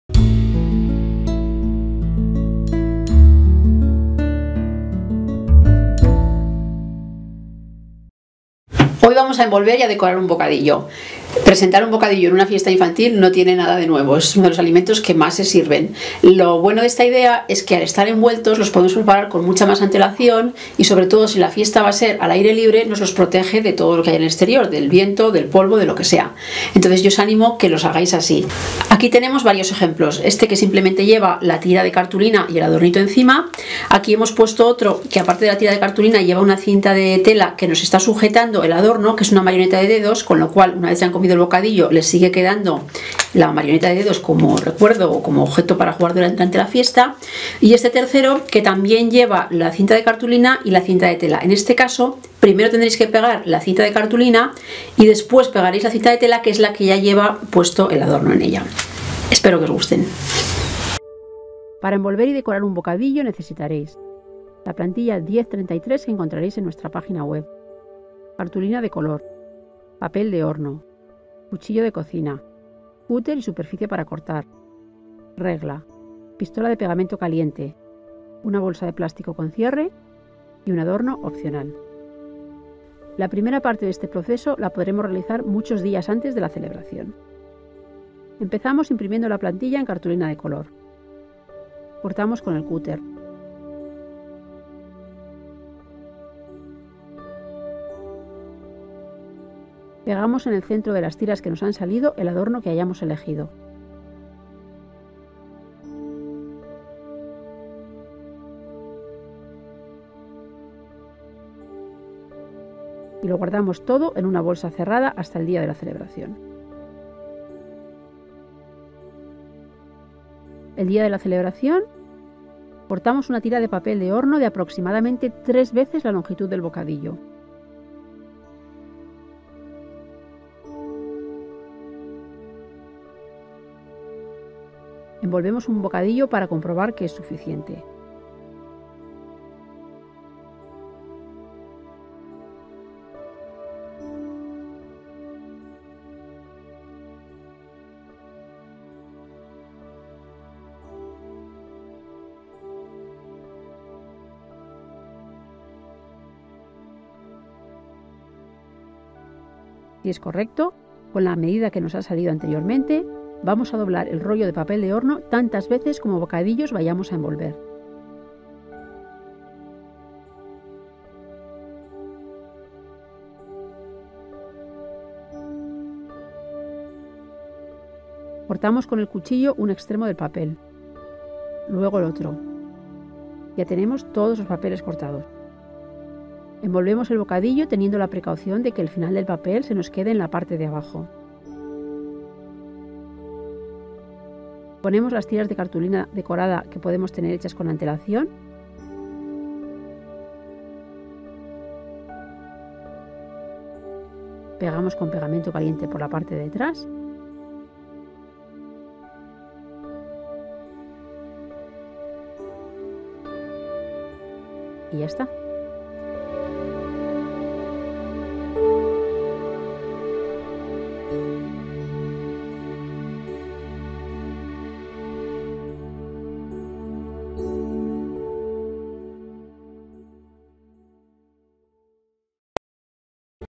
Cómo envolver y decorar un bocadillo
Si en una celebración vamos a servir un bocadillo como parte del menú para la merienda, siempre es bueno considerar el servirlo envuelto y decorado; nos permitirá prepararlo con un poco más de antelación, tendrá un aspecto acorde con el tema de la fiesta, y estará más protegido, una consideración muy a tener en cuenta cuando celebremos la fiesta al aire libre.